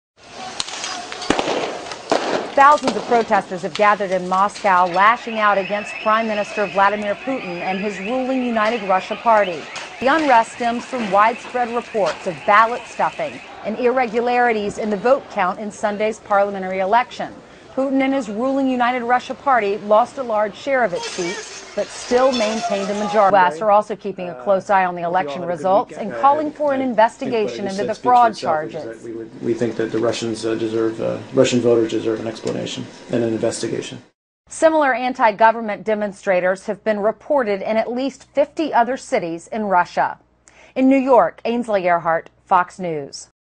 Fox News hopes you will never notice Russian protesters be dark-skinned!
Fox News deceived people giving the video about Greece riots for protests in Russia. Obviously, the USA want the same to take place in Moscow!